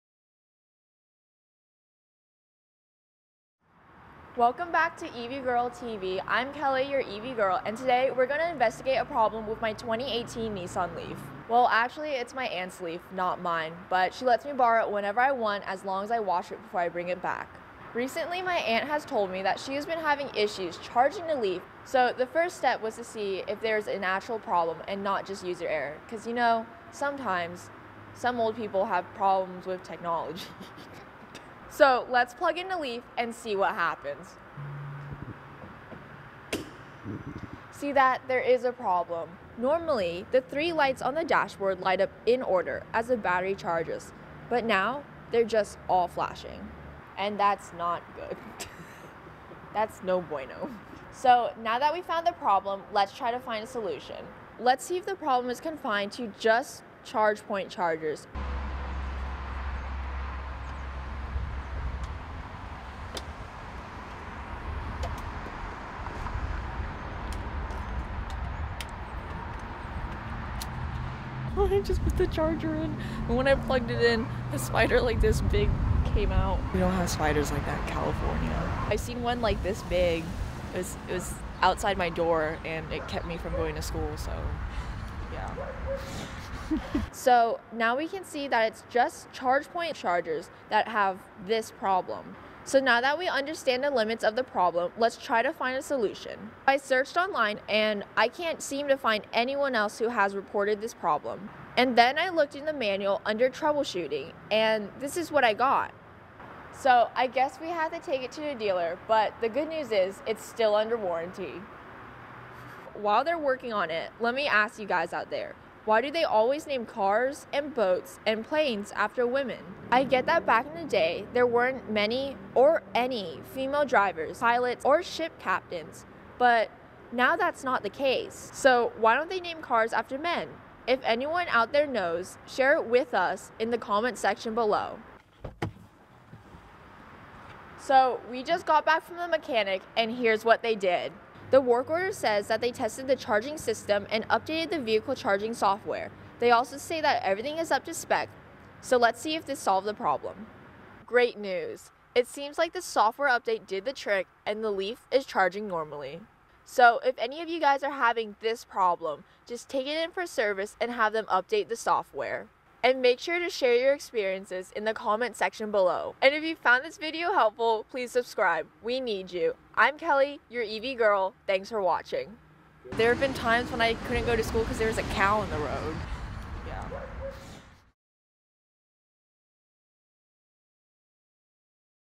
My 2018 Nissan Leaf Won't Charge!
My Aunt's 2018 Nissan Leaf won't charge at Chargepoint Level 2 chargers.

Watch the video to see what happens & how we fix it.

THX

Kelly
Your EVGRL